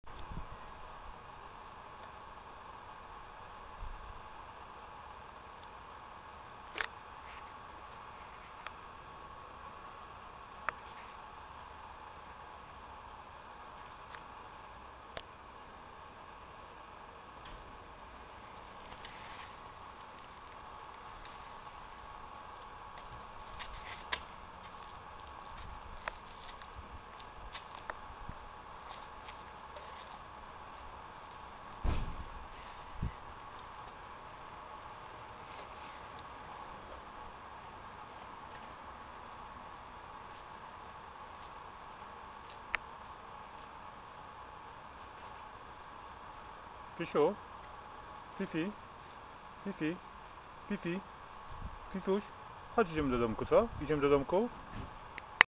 pinczer miniatura,miniature pinscher
Pinczerek fifi obserwuje okolicę
zdalna naprawa komputera